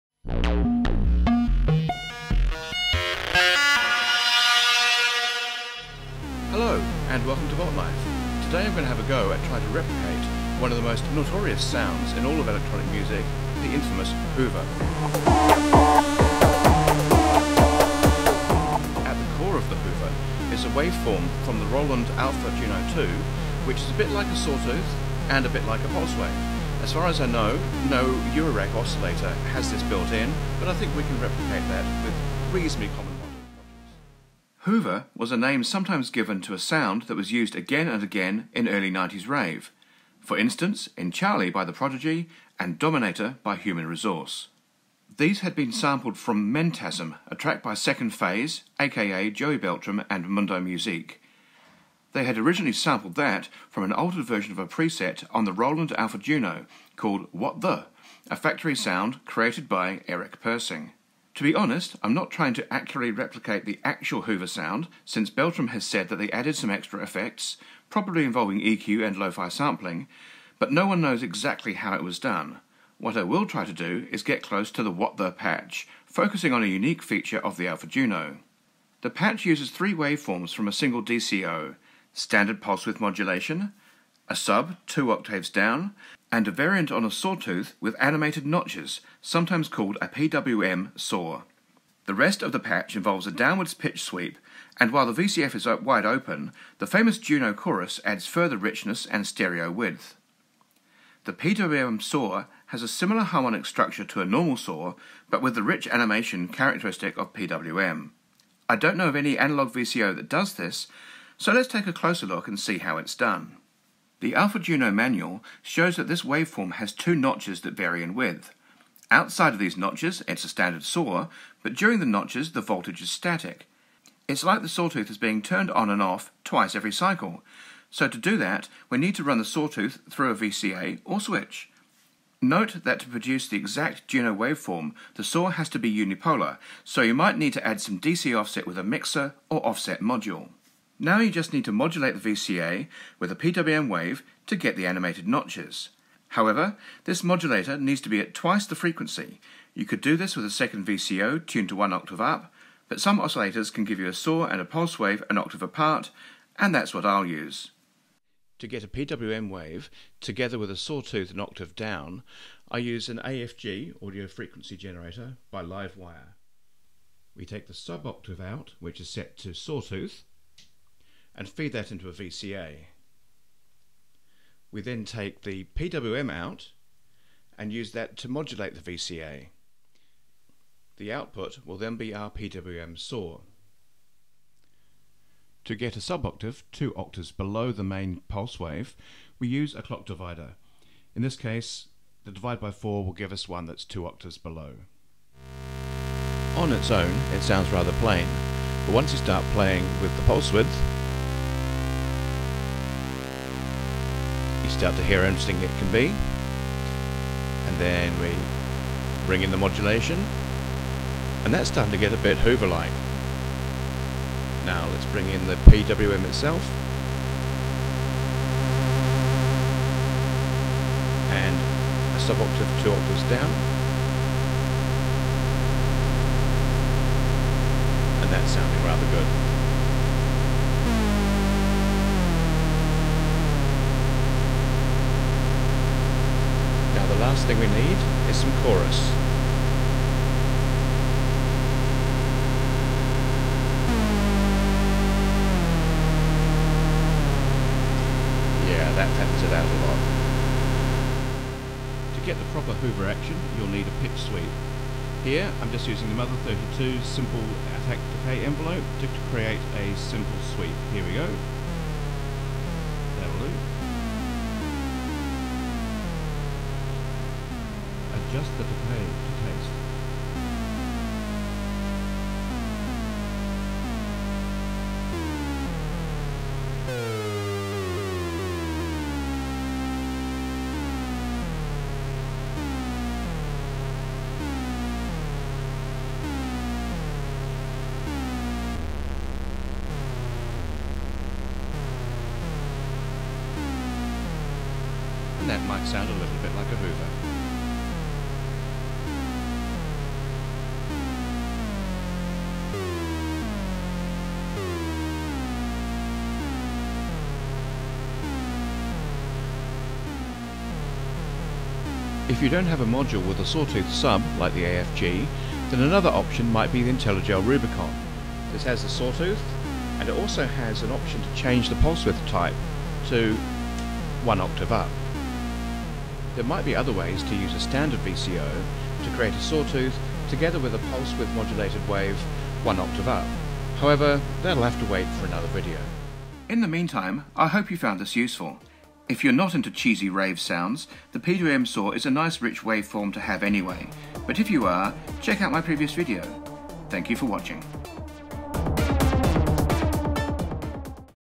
VOLTLIFE: How to Hoover
A tutorial on approximating the infamous “Hoover” sound in Eurorack, focusing on how to make a PWM Sawtooth wave like the Roland Alpha Juno.

A somewhat tetchy thread on how the alpha Juno creates the PWM Saw, with oscilloscope shots
Original “What The?” Patch settings